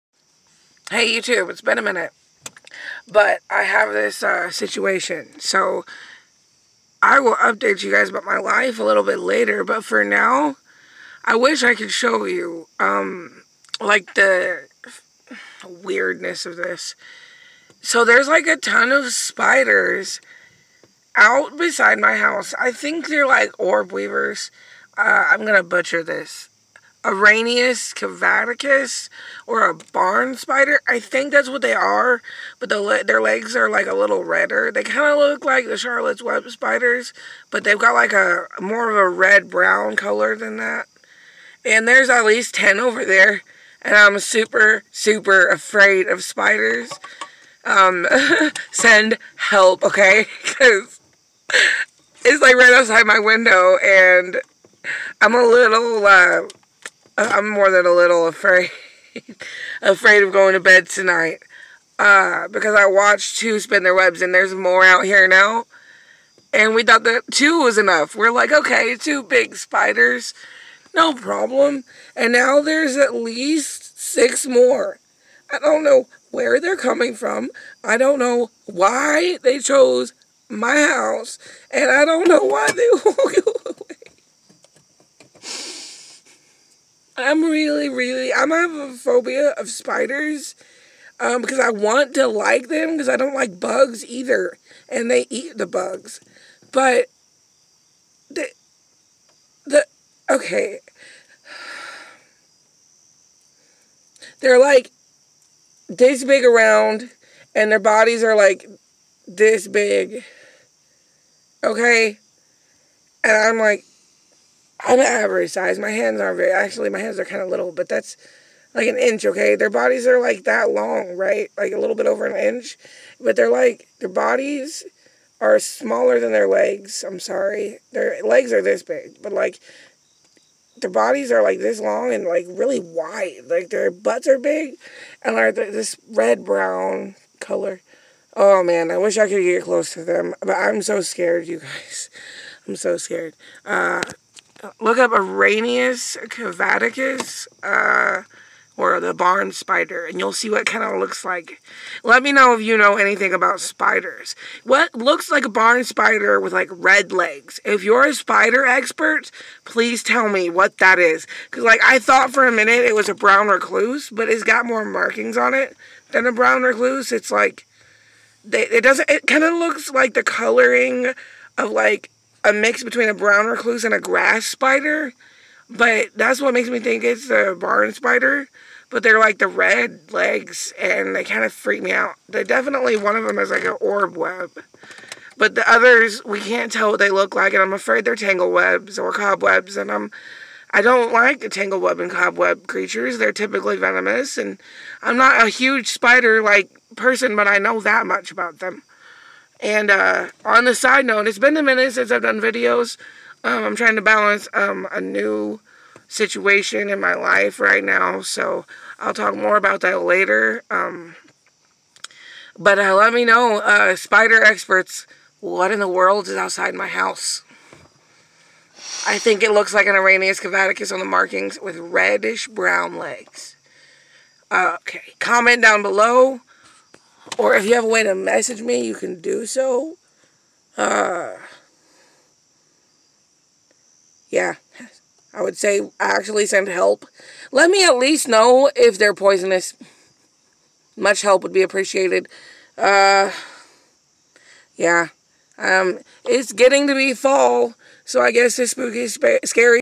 Spooky Scary Spiders
They are freaking me out. I will try to post a video of them later when it is light outside. It looks like an Araneus cavaticus, or barn spider, with reddish brown legs. eeek. Spider Experts, what are they? Sorry I'm too chicken to take photo and video of them right now. bagock.*flaps wings like a chicken*